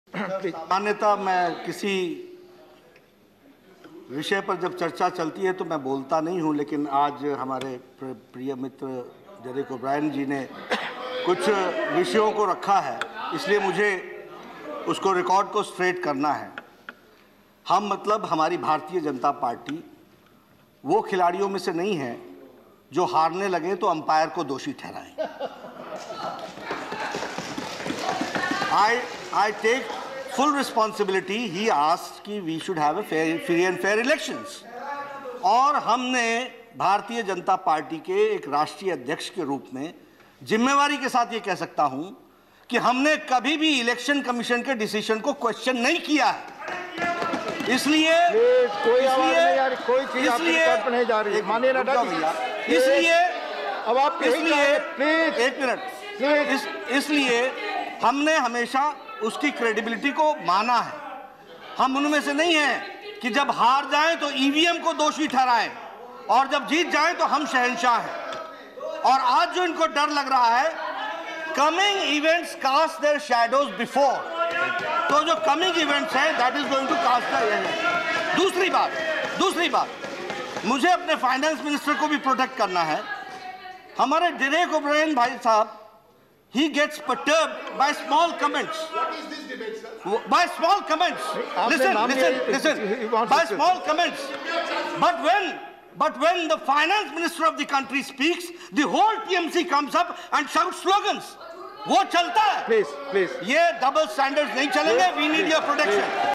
The BJP doesn't blame the umpire while losing. We've never questioned EC's decisions - Shri JP Nadda
The BJP doesn't blame the umpire while losing. We've never questioned EC's decisions - Shri JP Nadda in Rajya Sabha: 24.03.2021